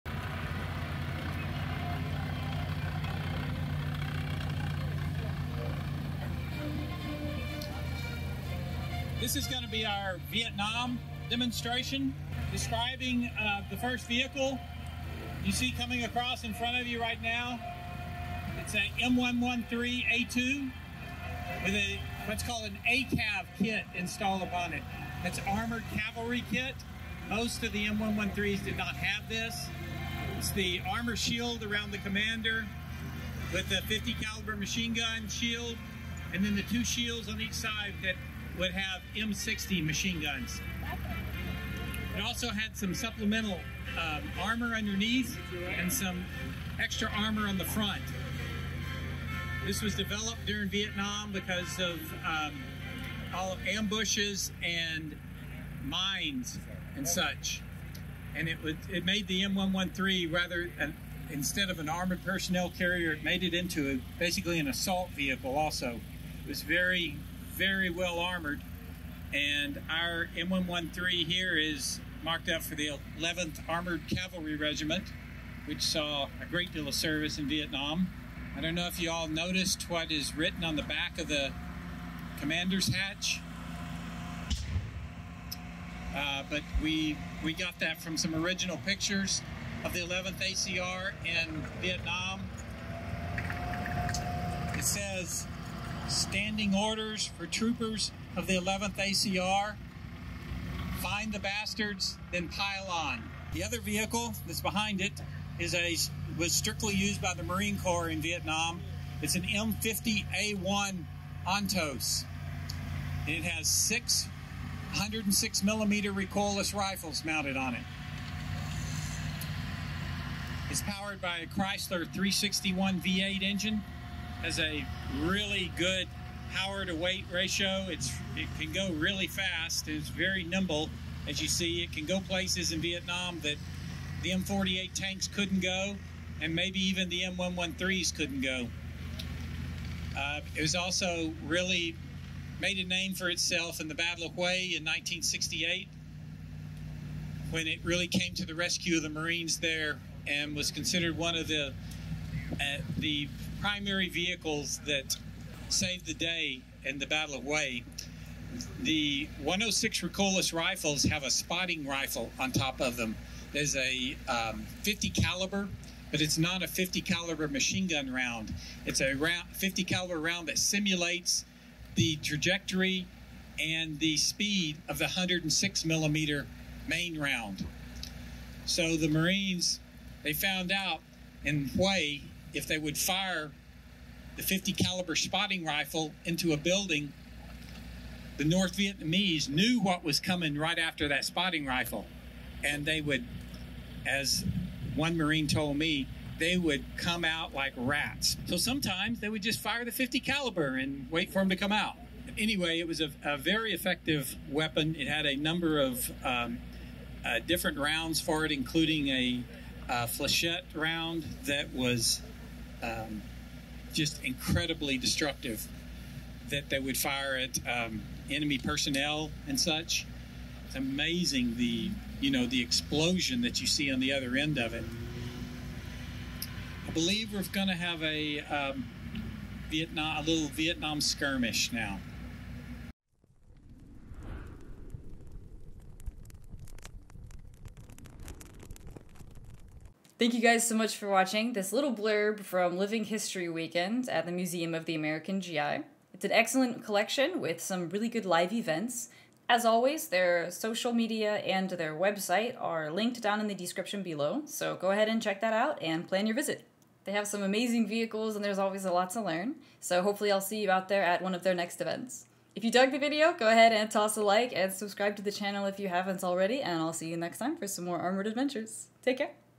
M113A2 and M50 Ontos at MAGI Living History Weekend
Join the crowd and I as MAGI Owner Brent Mullens gives a short show-and-tell of two beautifully restored Vietnam era armored vehicles from his collection.

Thank you to MAGI for the preservation of these vehicles and the stories that attend them.

Thank you to all Vietnam Era Veterans for your service. You are appreciated and will never be forgotten.